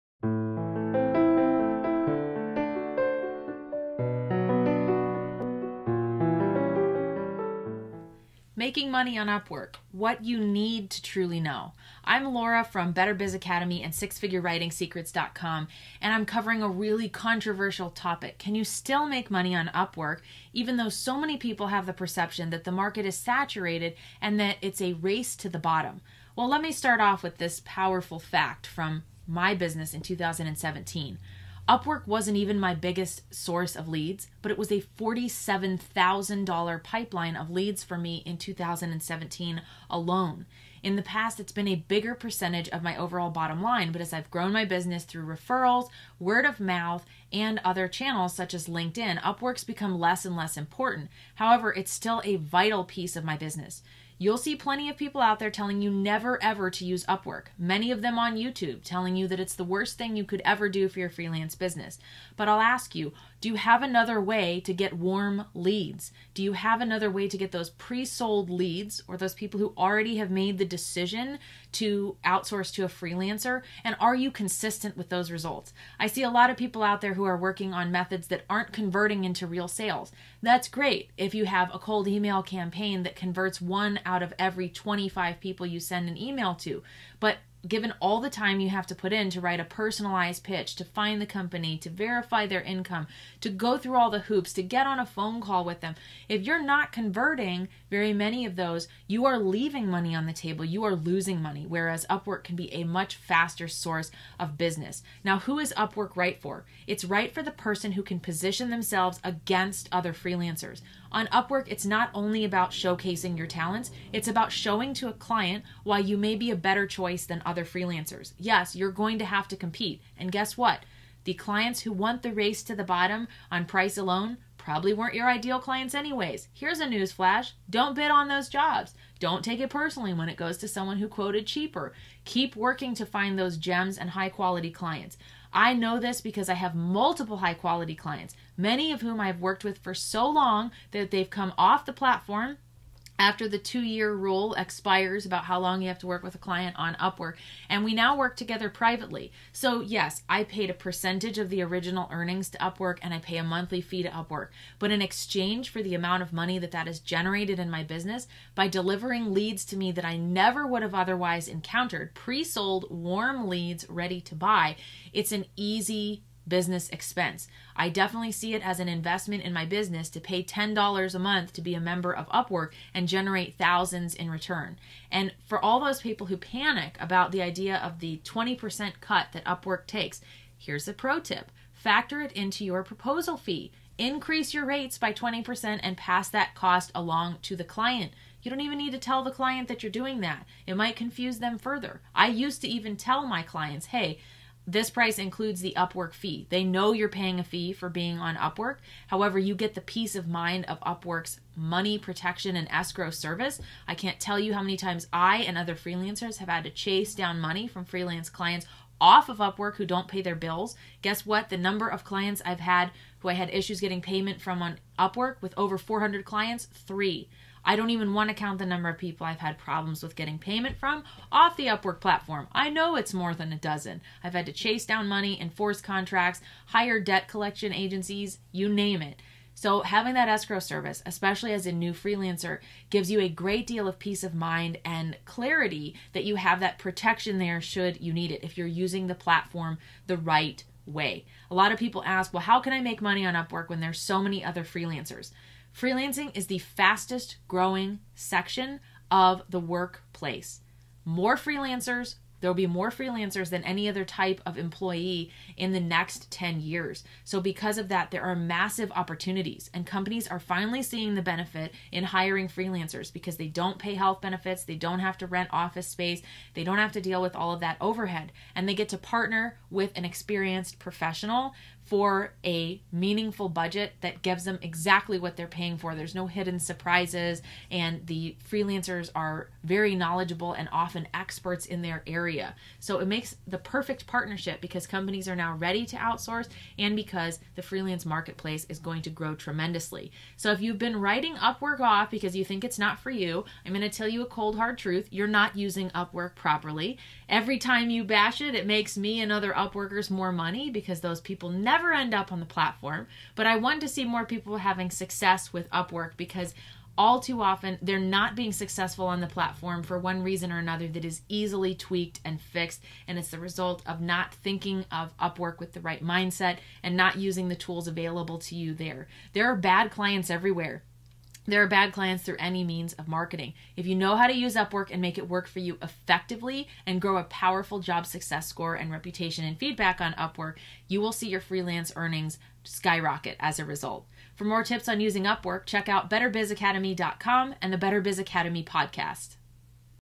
Upwork was a $47k Pipeline in 2017 - How to Make It Work for You
I'm covering a really controversial topic: Can you still make money on Upwork even though so many people have the perception that the market is saturated and that it's a race to the bottom. Let me start off with this powerful fact from my business in 2017. Upwork wasn't even my biggest source of leads but it was a $47,000 pipeline of leads for me in 2017 alone.